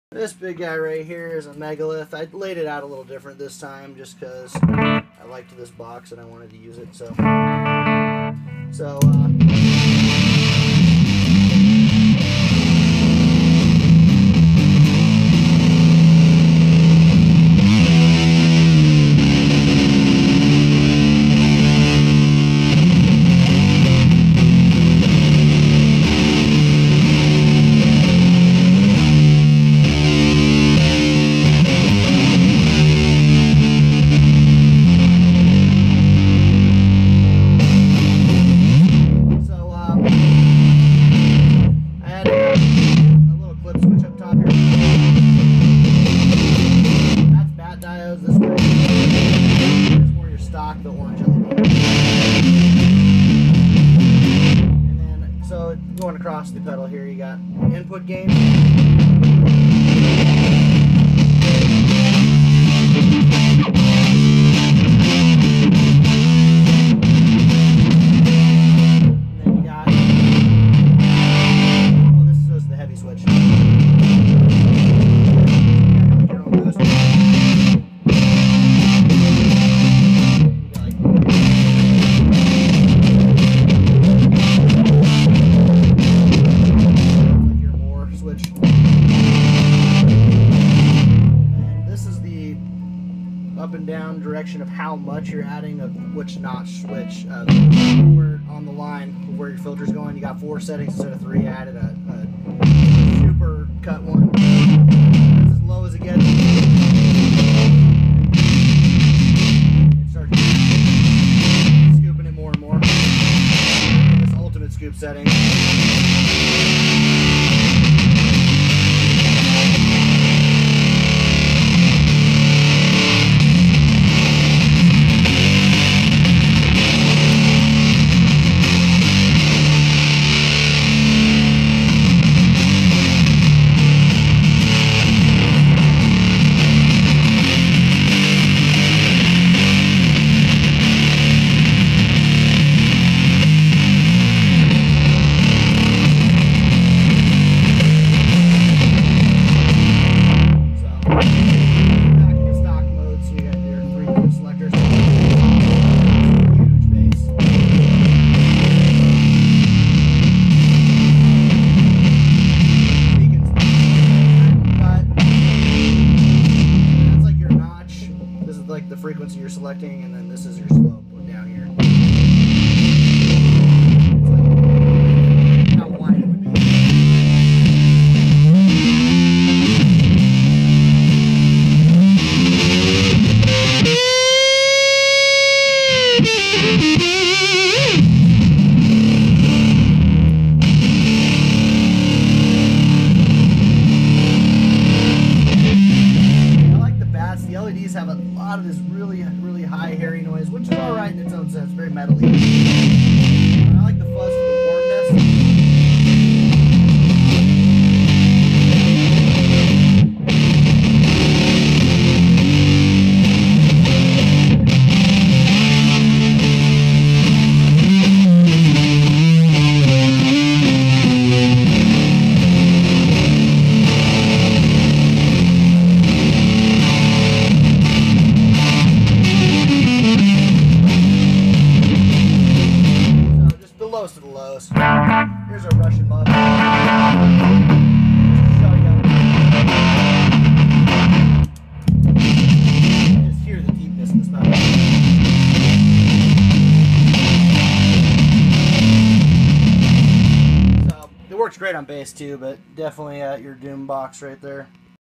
Megalith Built By Ryan Fuzz with mods. Doom Pedal
Doom muff varient fuzz. Added a clip switch up top for led and BAT, also added a forth setting on the rotary for less bass to get even more range out of the beast will make it fun to layer in recording.